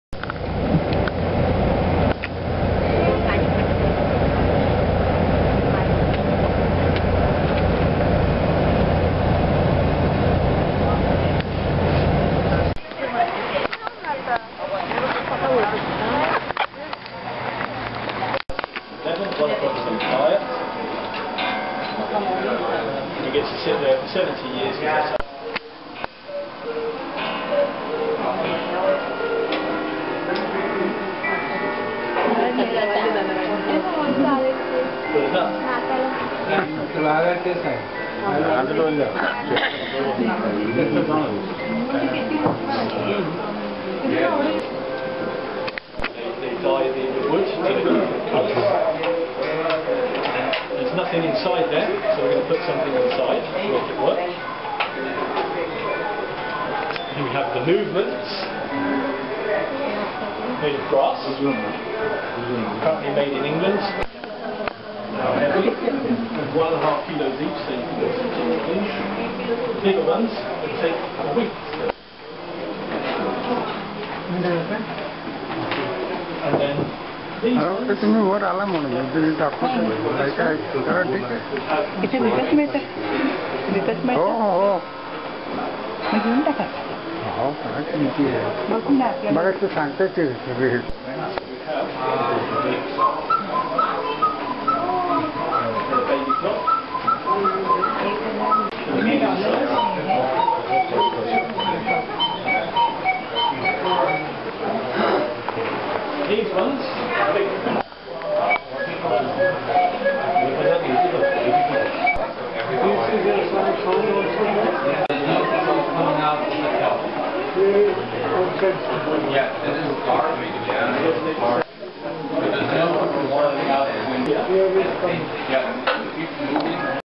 Cuckoo Clock Factory at Drubba,Titsee Switzerland 2013
Fabulous to watch different aesthetic models of clocks in the factory.Live demonstration of manufacturing the clock was interesting.